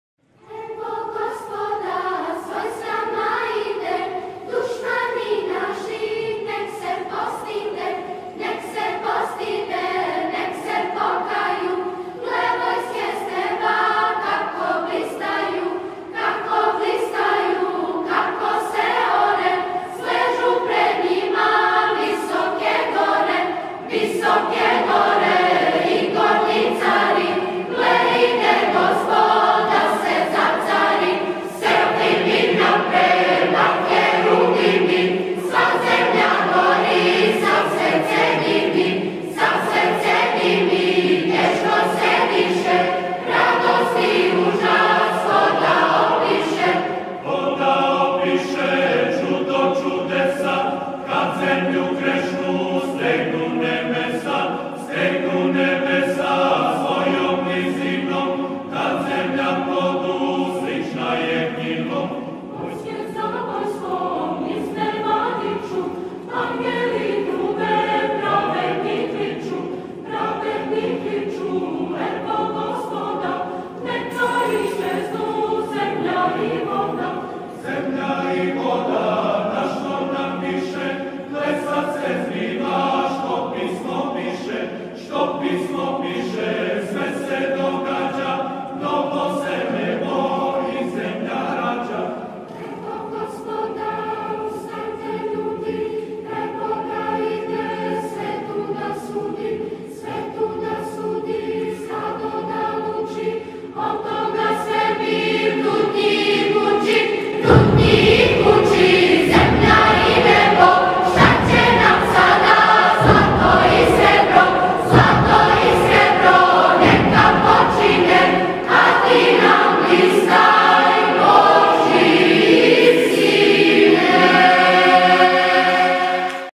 ✝ The Terrifying Judgement ✝ When I heard this song about the Second coming of Jesus, I got chills 🥶
Choir of the Serbian Orthodox Church chants to the most glorious, joyous and horrific day to come, the Second Coming of our Lord Jesus Christ.